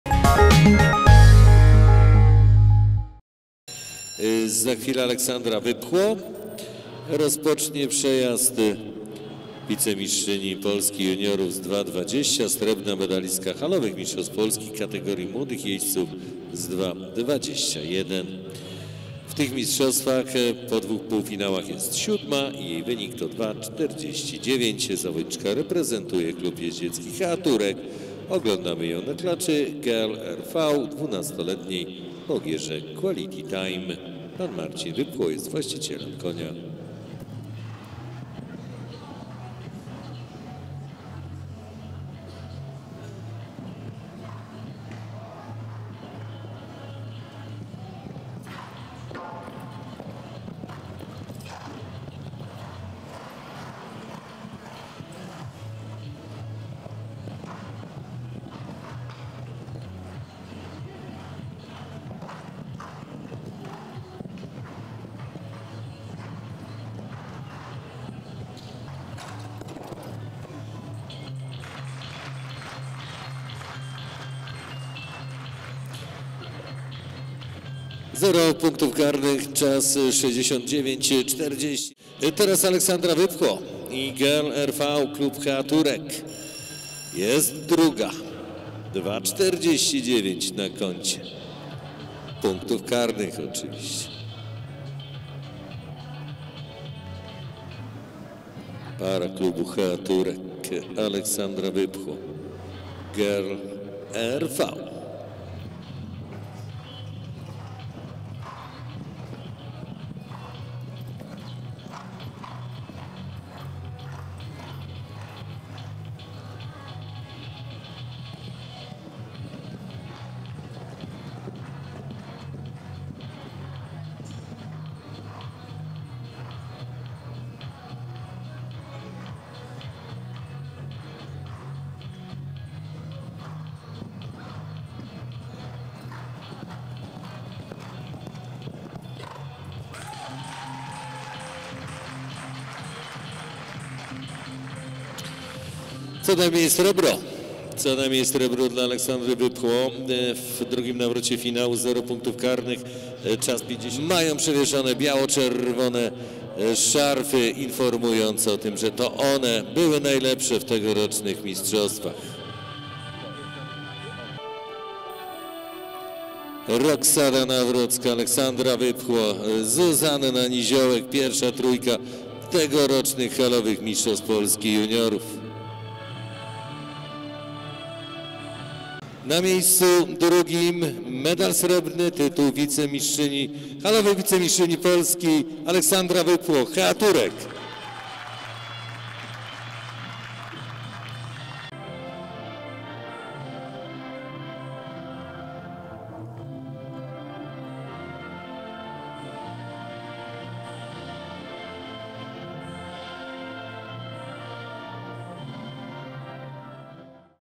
Kolejne srebro Oli Wypchło z HEEA Turek na Halowych Mistrzostwach Polski
To kolejne srebro w dorobku juniorki, trenującej i startującej od kilku lat w Klubie HEEA Turek, do którego zaprosili ją jego właściciele, państwo Opłatkowie. Ola nie ukrywa, że jest im przeogromnie wdzięczna. – To dla mnie naprawdę świetna sprawa móc stacjonować w najlepszym ośrodku w Polsce, ze świetnymi ludźmi wokół i całkiem nowa jakością treningów, tu na miejscu bez męczących dojazdów do Konina, Poznania czy Łodzi, gdzie trenowałam wcześniej – podkreśla.